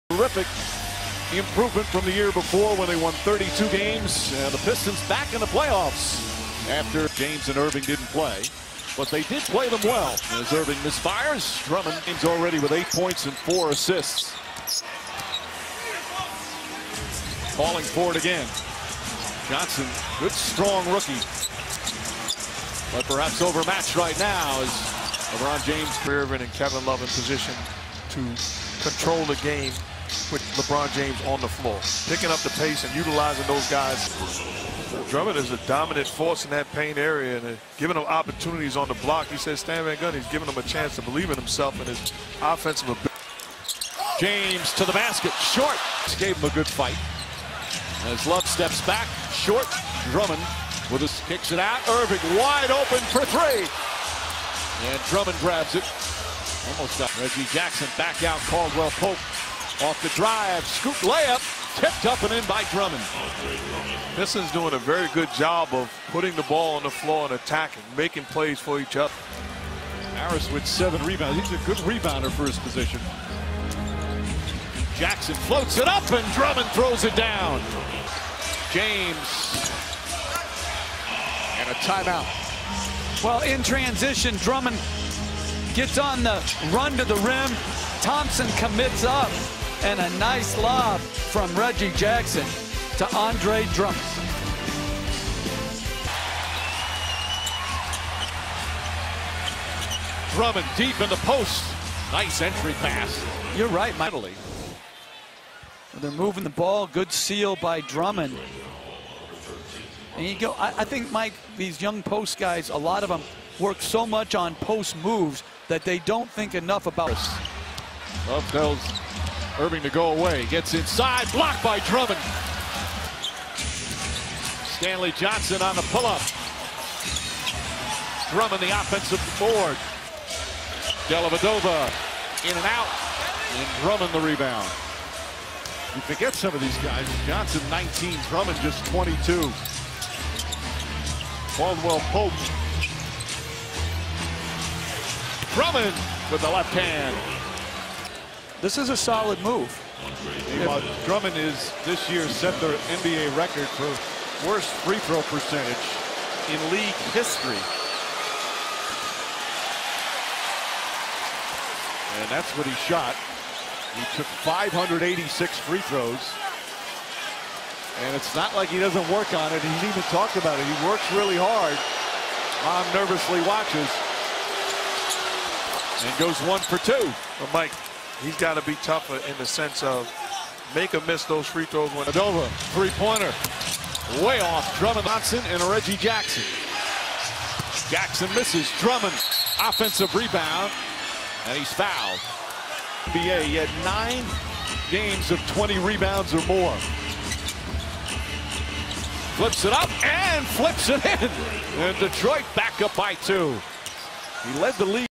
Andre Drummond Full Game Highlight VS Cleveland Cavaliers(13Points,11Rebounds)
NBA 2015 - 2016 Playoff Game
Eastern Conference 1ST Round - Game1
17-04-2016
Detroit Pistons VS Cleveland Cavaliers